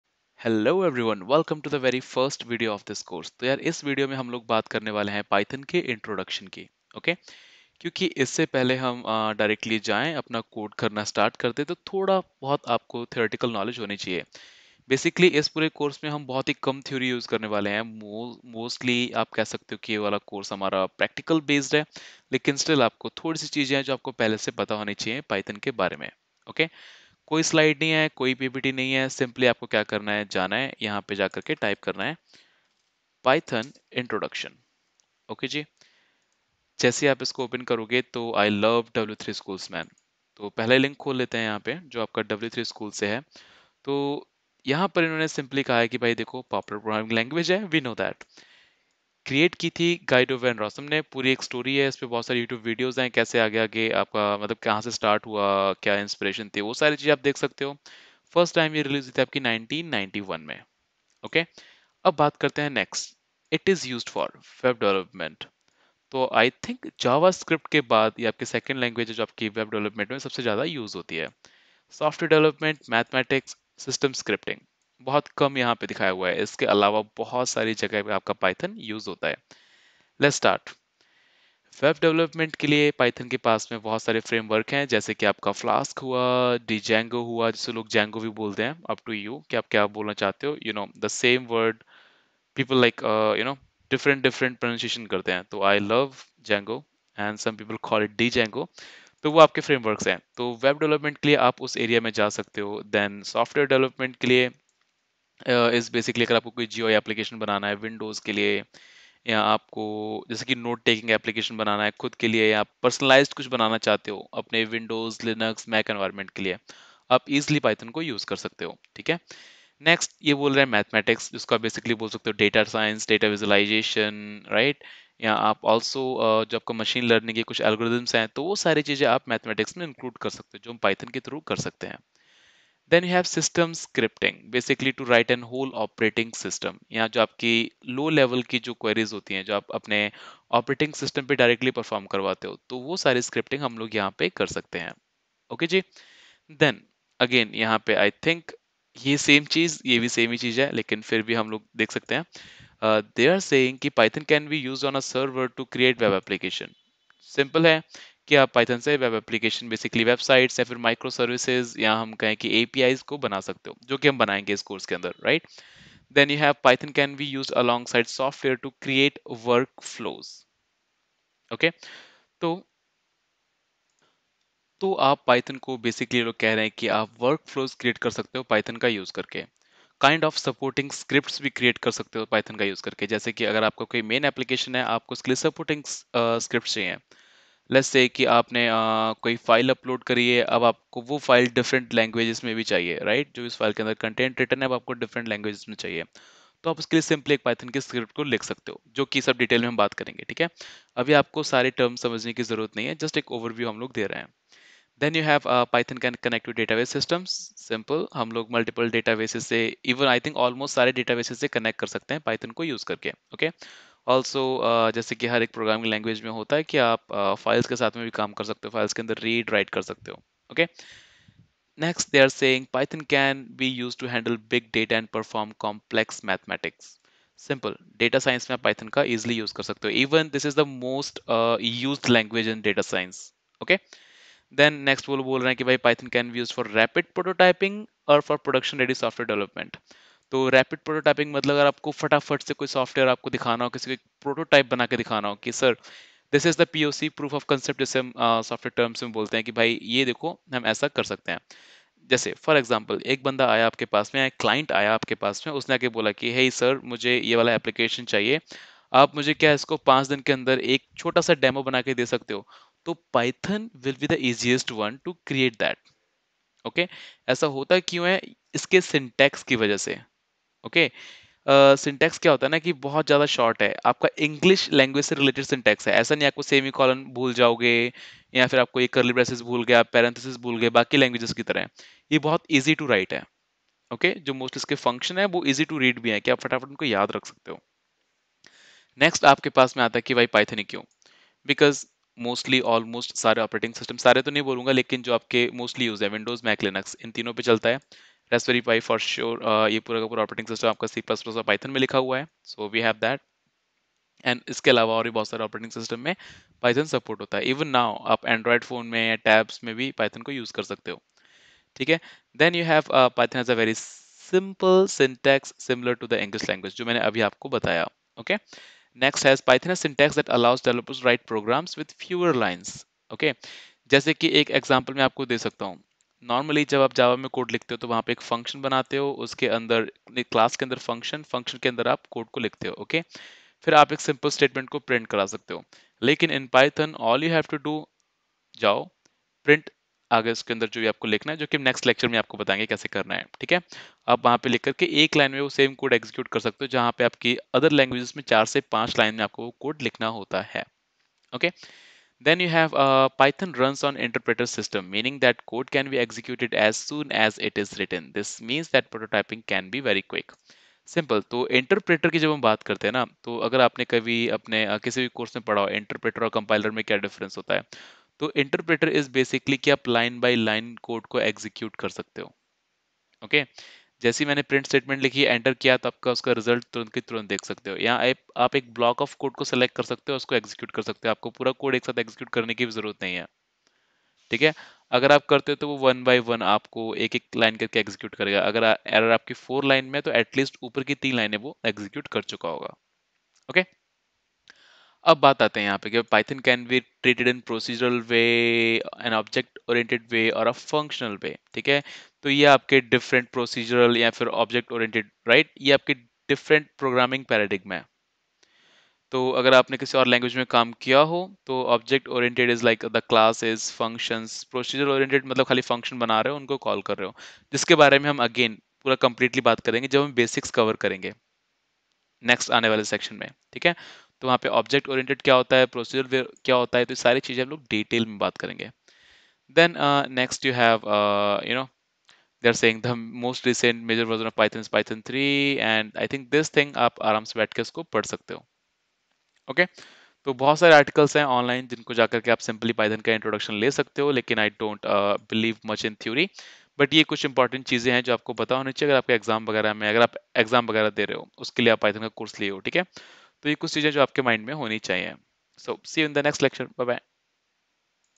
1. Introduction to Python | Python in Hindi
This is the first video in our course Python in Hindi (2022) series. Which will have all the latest videos related to Python.